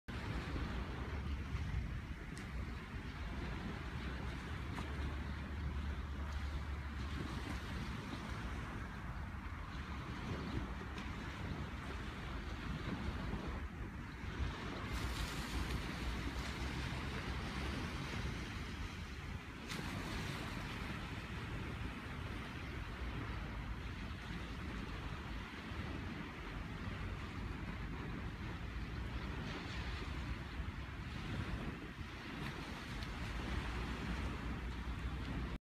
Amanecer de junio en El Campello
Precioso día de casi verano en El Campello. 27ºC y el mar en calma ¡Os esperamos!

No hay nada más relajante que detenerse unos instantes a escuchar el sonido de las olas al llegar a la playa ¿Verdad? El mejor modo de comenzar el día.

Imágenes captadas en la Playa de Muchavista, El Campello (Alicante, Costa Blanca, Comunidad Valenciana)